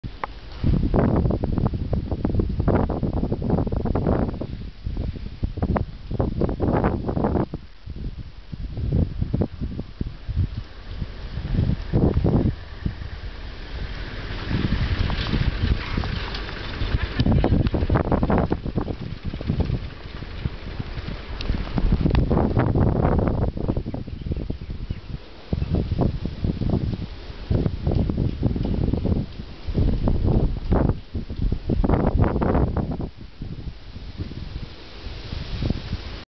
Anomalie Kilka snieżyc w Kwietniu.
Tego typu śnieżyc przetoczyło się kilka w tym miesiącu!